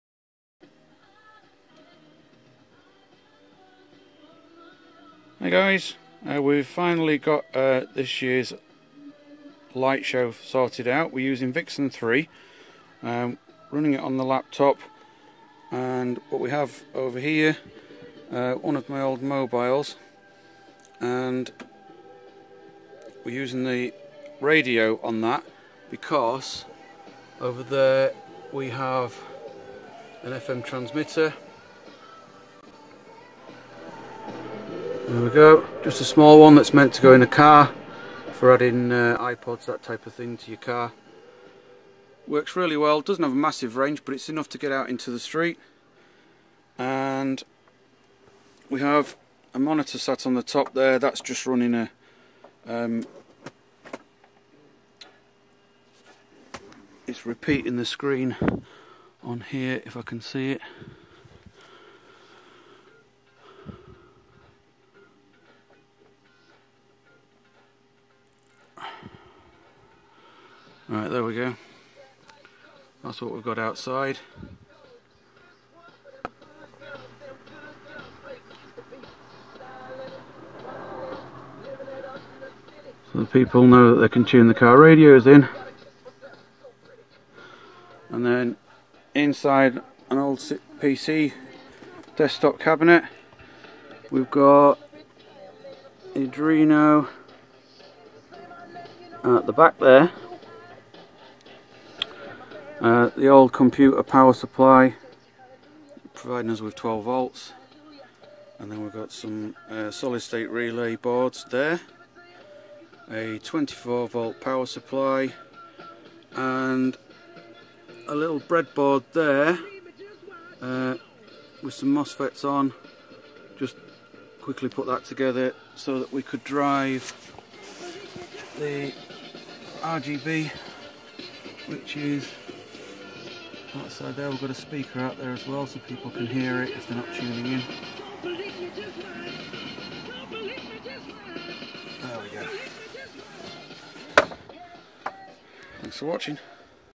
Arduino DIY LED christmas lights controller 2014 version 1
Our first attempt at an arduino controlled christmas light show using vixen 3 software